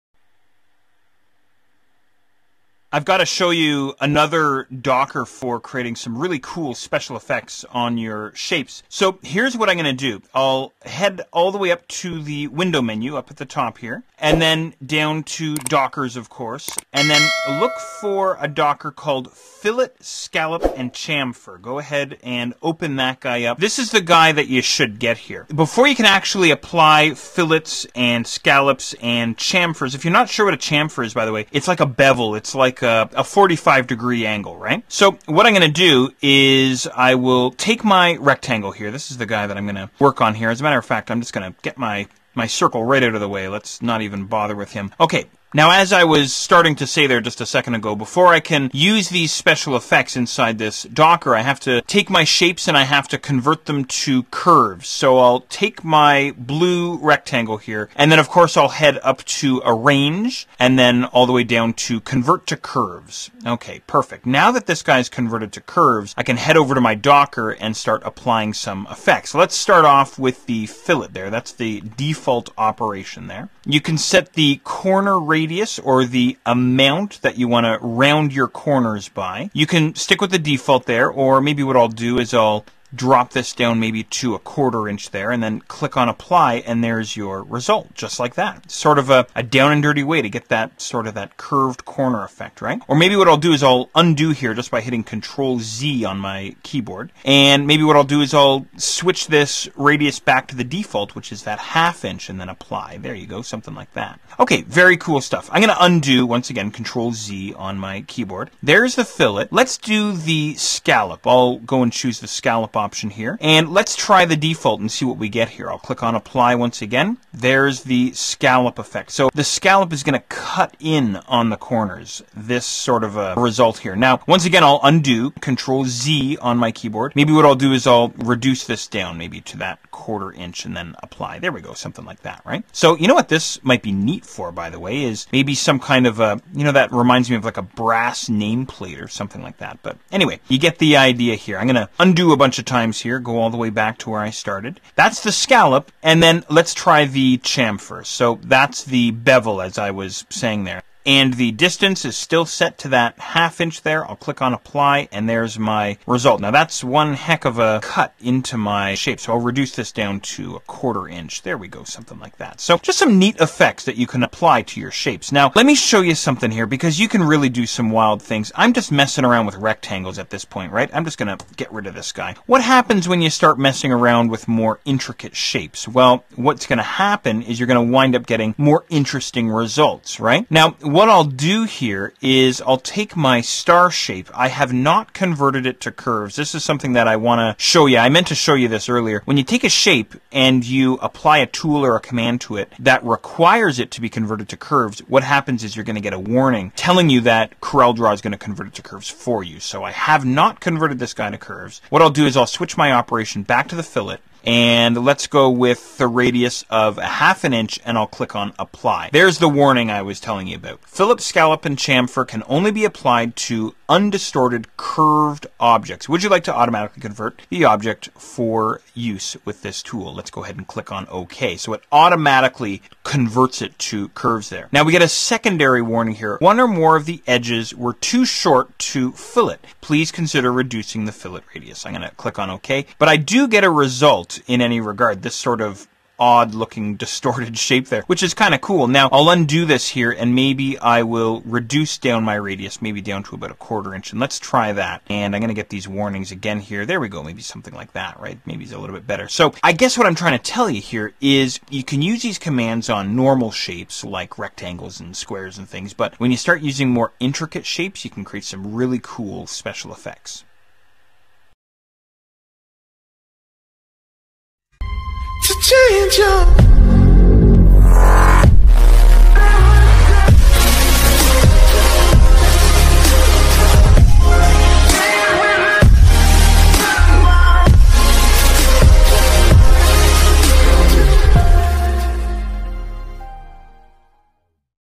Applying Fillet Scallop & Chamfer | CorelDRAW Graphics Suite X4 - WUZZZAPP
The filleted corner effect applies uniform and concentric roundness around a point at a specific radius, enabling you to convert a sharp corner to a rounded corner. The filleted corner is a common design effect. The scalloped corner effect instantly applies concentric reverse-rounded corners to objects that result in a seashell-like effect, which is essentially the opposite of the filleted effect. Like the fillet command, the scallop command can be applied at specific radius values. Although not as commonly used as the rounded corners, the scallop effect is equally as useful in eliminating the time required to manually create similar effects. In the example shown below, certain objects in this arrangement were given scalloped corner effects along with alternating bitmap fills to create a wood pattern tile.